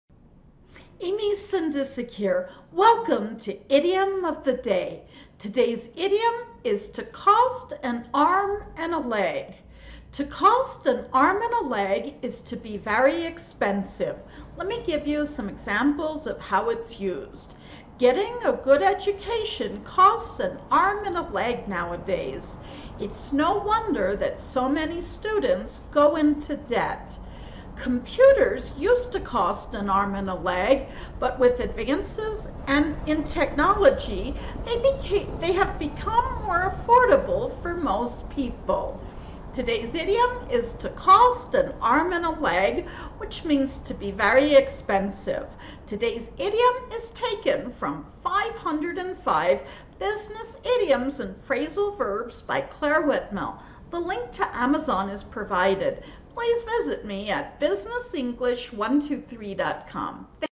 IDIOM OF THE DAY cost an arm and a leg
IDIOM OF THE DAY is featured daily from Monday to Friday and presents a Business English idiom with examples.

Today's idiom is:  cost an arm and a leg

If you want to know more about my courses, you can visit me @

Whitmell, Clare (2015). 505 Business Idioms and Phrasal Verbs